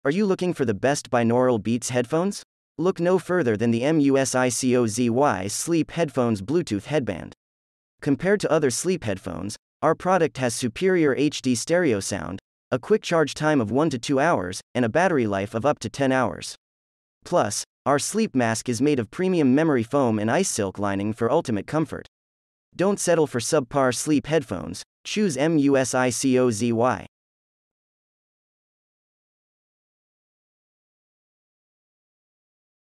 Best Binaural Beats Headphones: MUSICOZY Sleep Headphones Bluetooth Headband
Are you tired of tossing and turning at night? Do you struggle to fall asleep? Look no further than the MUSICOZY Sleep Headphones Bluetooth Headband. Our product is the best binaural beats headphones on the market, and we'll show you why.

Compared to other sleep headphones, the MUSICOZY Sleep Headphones Bluetooth Headband has superior HD stereo sound, providing you with a high-quality audio experience that will transport you to a world of relaxation and calm. Our product also has a quick charge time of just 1-2 hours, and a battery life of up to 10 hours, so you can enjoy uninterrupted sleep throughout the night.

Our sleep mask is made of premium memory foam and ice silk lining, ensuring ultimate comfort while you sleep. The MUSICOZY Sleep Headphones Bluetooth Headband is perfect for those who struggle to sleep in a noisy environment, or for those who want to enhance their sleep quality.

Don't settle for subpar sleep headphones. Choose the MUSICOZY Sleep Headphones Bluetooth Headband and experience a peaceful, restful sleep like never before. Click through to buy our product and transform your sleep experience today.